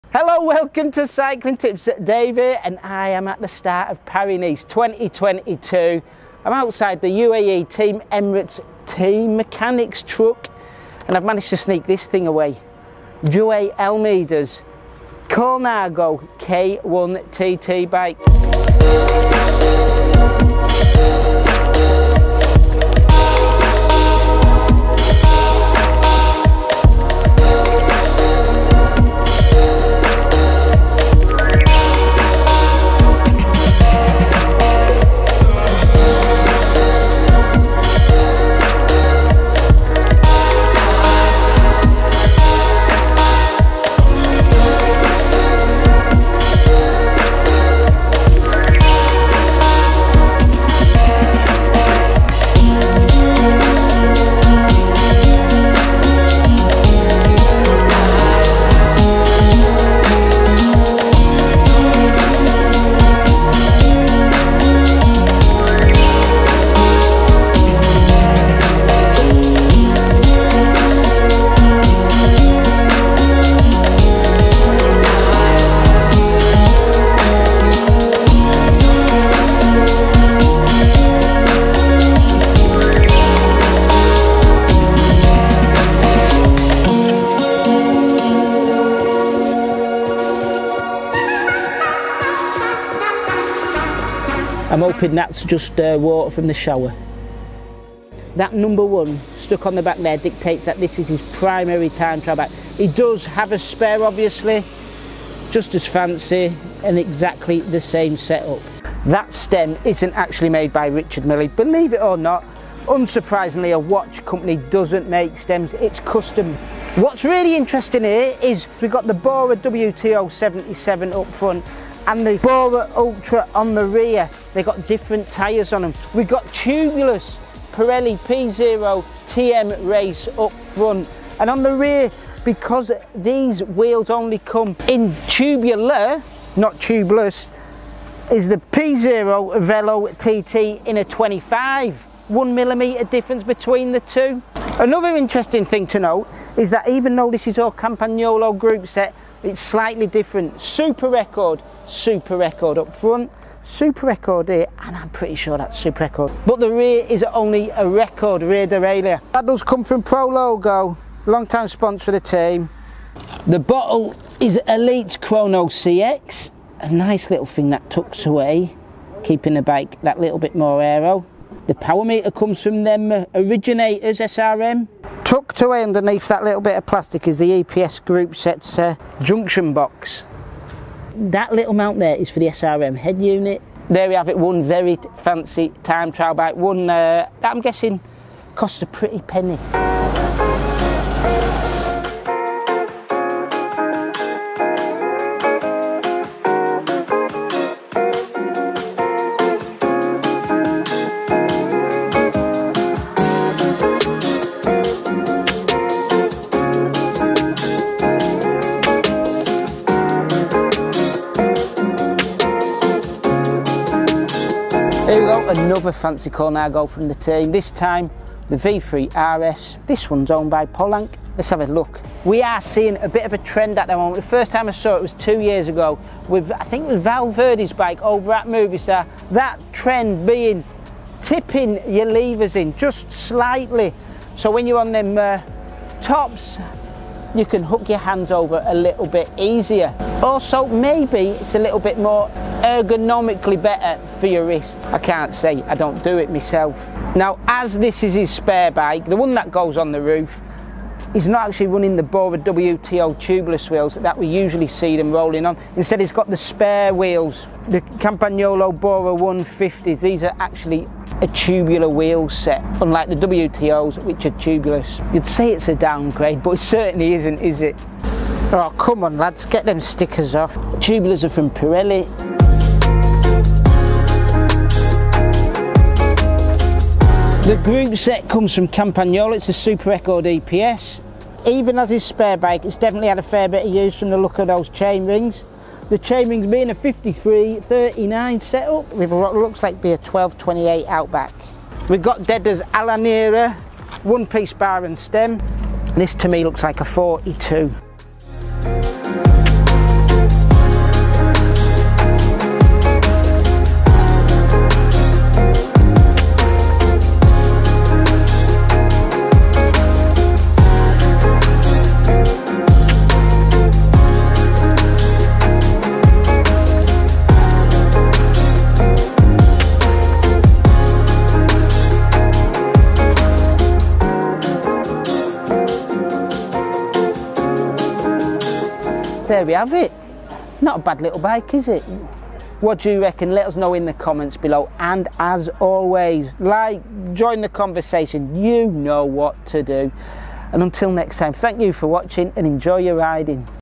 Pro Bikes: UAE-Team Emirates' Colnago V3RS & K.ONE TT bike
We managed to wrestle two team bikes away from the UAE Team Emirates mechanic's truck at the recently held, Paris-Nice stage race. The K.ONE time trial bike and the V3RS road bike, both from Colnago are both perfectly kitted out in all Italian equipment. But surprisingly,  it's not all "the best of the best", even for team leaders, when it comes to time trial components.
Please delve in and see what we mean.